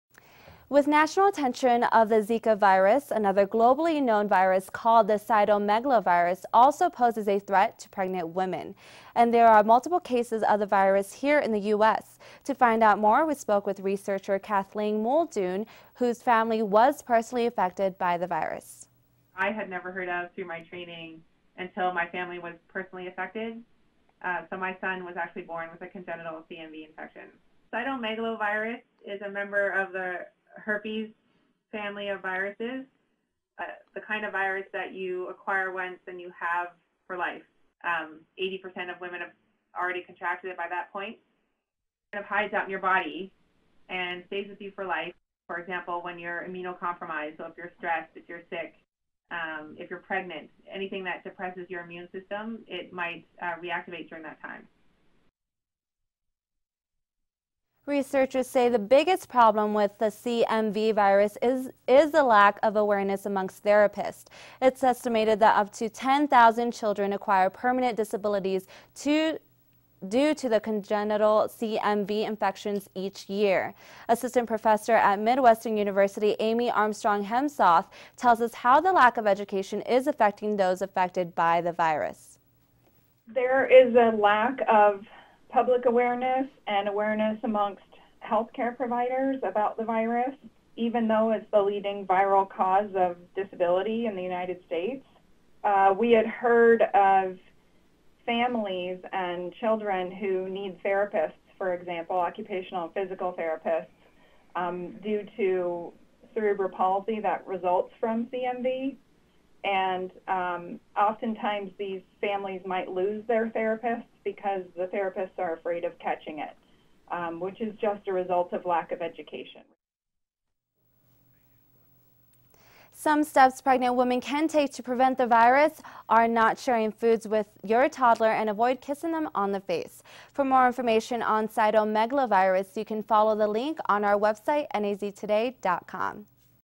CMV: A virus with little public knowledge affects Pregnancies
Cytomegalovirus (CMV) is a member of the herpes family. Many women are carries form of  CMV. The virus is suppressed until the  immune system is compromised such as in a pregnancy causing it to reactivate.

NAU assistant professor Kathleen Muldoon had very little knowledge of the virus, until she and her family were personally affected by it.  Muldoon's son was born with congenital CMV. He will continue to have complications from it his entire life.

Amy Armstrong-Heimsoth Assistant Professor at Midwestern University says the main issue with CMV is the lack of education surrounding it. Due to the little knowledge surrounding the disease families can lose physical therapists because fear of contracting the disease.

Featured in this story NAU Assistant Professor Kathleen Muldoon and Assistant Professor at Midwestern University Amy Armstrong-Heimsoth